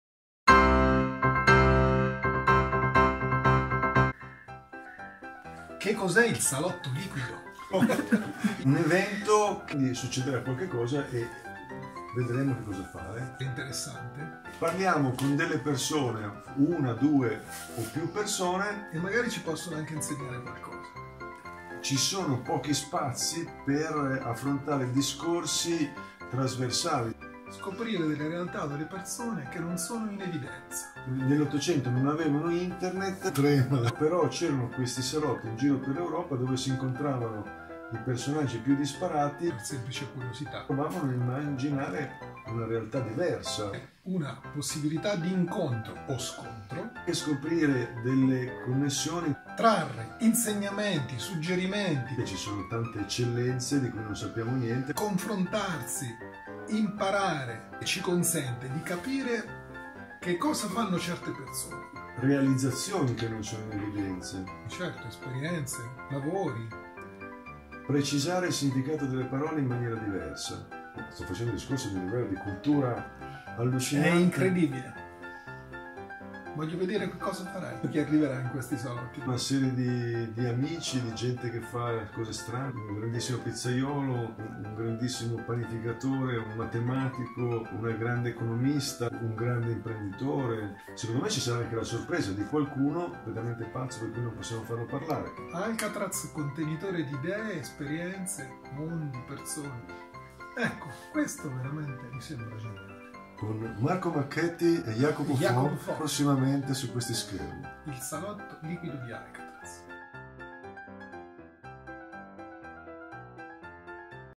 IL SALOTTO LIQUIDO DI ALCATRAZ - PROMO
Jacopo Fo e Marco Marchetti presentano: il salotto liquido di Alcatraz.
Per maggiori informazioni CLICCA QUI

MONTAGGIO E RIPRESE: Alcatraz Channel